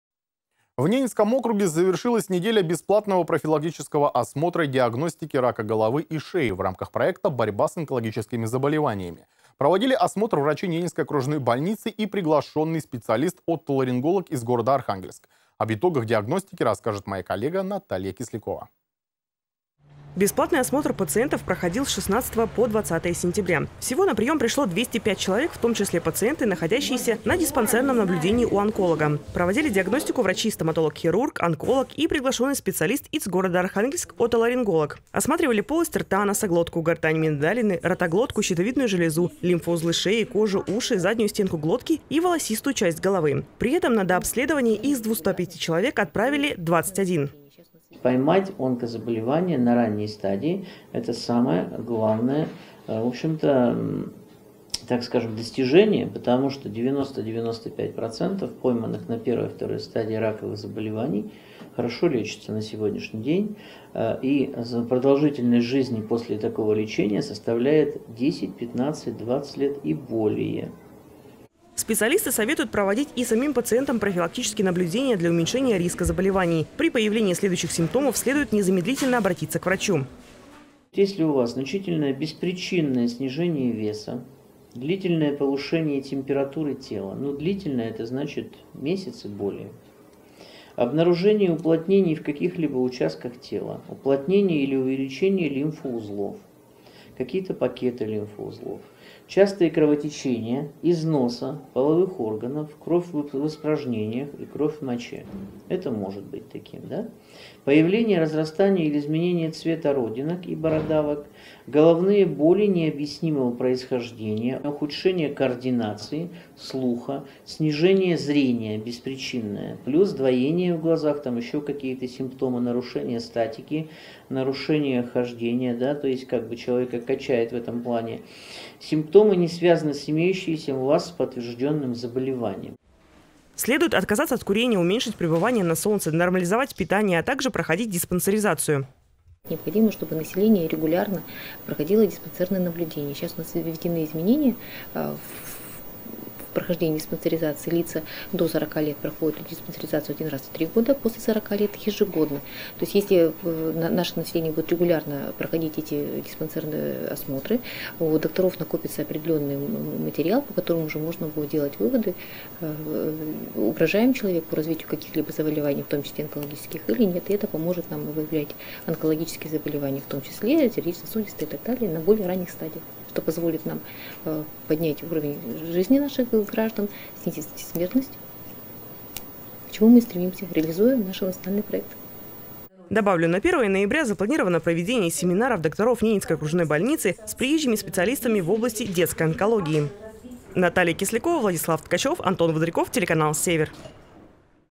ПРОФИЛАКТИКА ОНКОЗАБОЛЕВАНИЙ
В Ненецком округе завершилась неделя бесплатного профилактического осмотра диагностики рака головы и шеи в рамках проекта "Борьба с онкологическими заболеваниями". Проводили осмотр врачи Ненецкой окружной больницы и приглашенный специалист отоларинголог из города Архангельск.